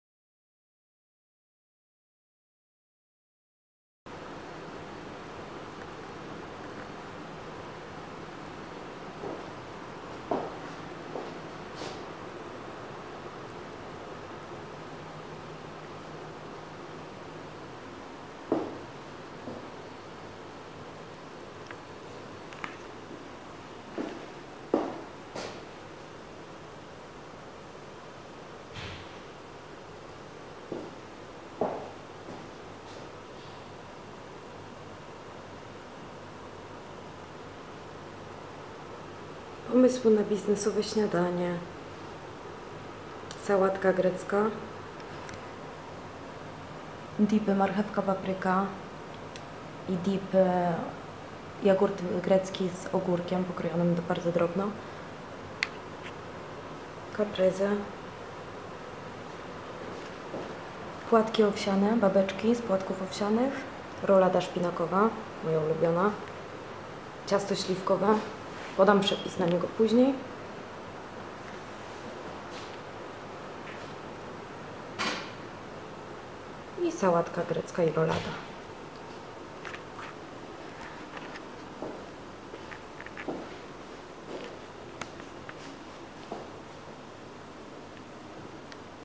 Propozycje dań: catering, lunch, urodziny, przyjęcie, biznesowe śniadanie
Więcej na:
(przepisy, zdjęcia)

W pracy zostałam poproszona o zorganizowanie i przygotowanie biznesowego śniadania. Mottem przewodnim było hasło „fit śniadanie„, dlatego mnie poprosili o organizację tego wydarzenia. Miałam ograniczony budżet na około 30 osób miałam do dyspozycji 120 zł. Jeśli zrobicie zakupy z głową za 120 zł możecie kupić wiele produktów i wbrew pozorom zrobić sporo dań. Zakupy zrobiłam w biedronce, zapłaciłam około 114 zł + osobno kupiłam pieczywo. Przygotowałam menu, miałam dużo za dużo pomysłów. Postanowiłam zrobić szwedzki stół, do tego sałatki, roladę, przekąski „na już”.